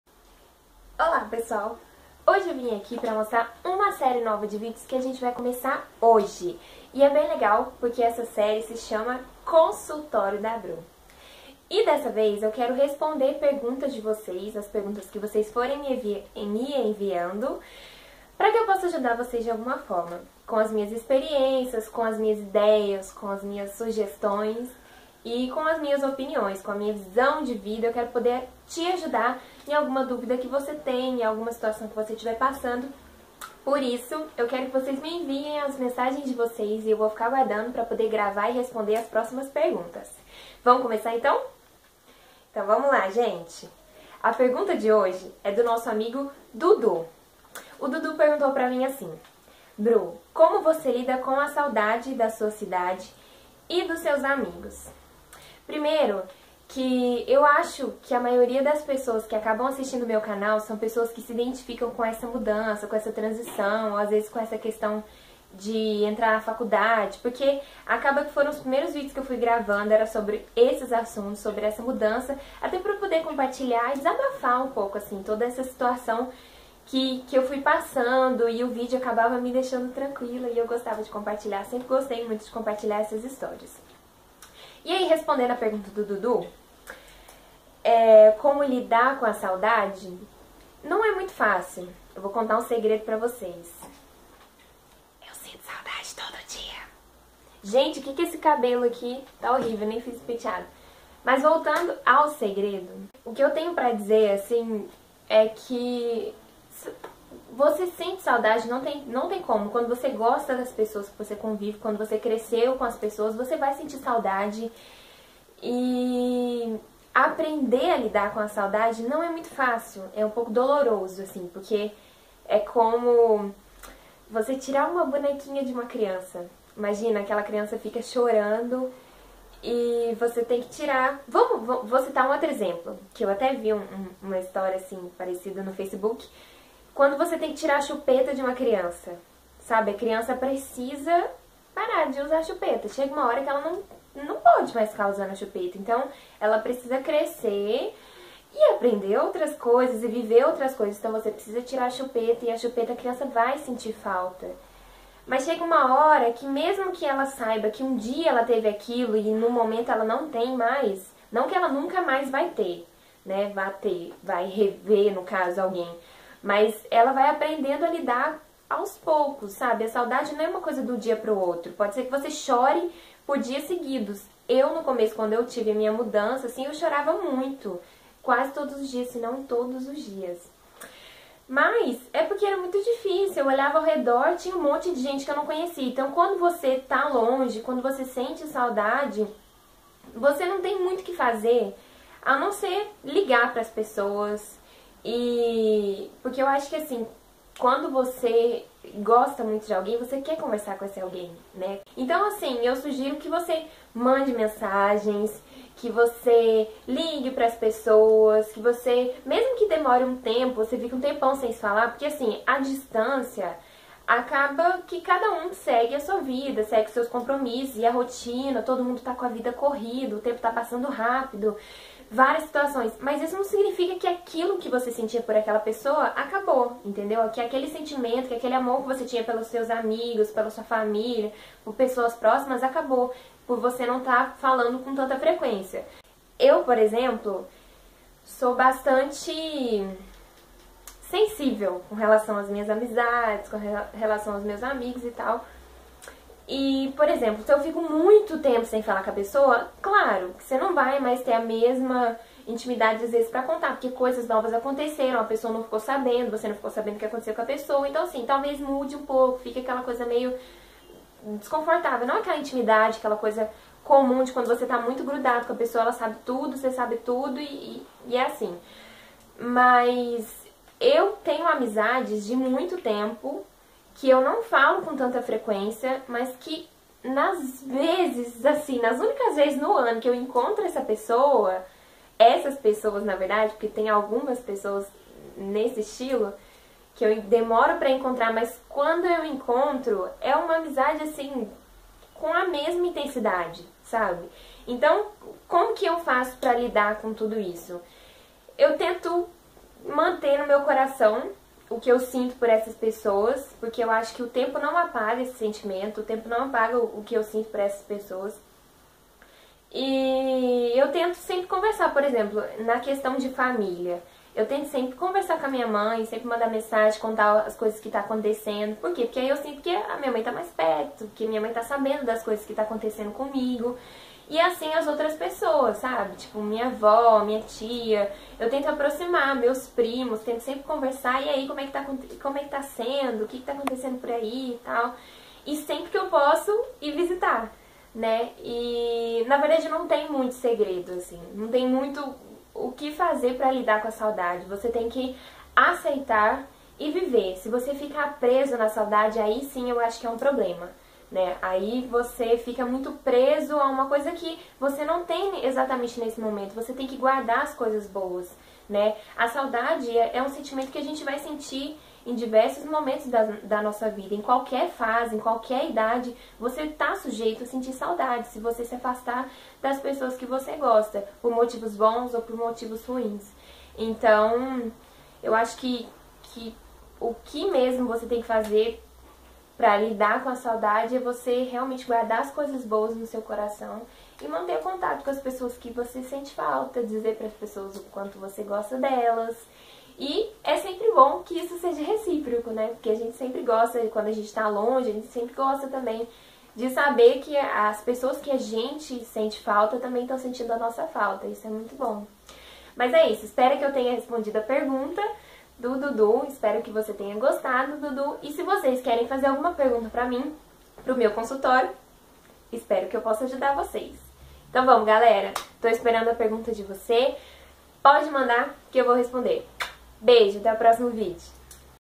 Como lidar com a saudade
Nova série de vídeos respondendo perguntinhas e tentando ajudar vocês! Não esqueça de se inscrever aqui no canal e dar um joinha no vídeo.
Fico aguardando as questões de vocês, e podem mandar em qualquer rede social com a hastag, ou poste aqui no canal mesmo!

beijooooos!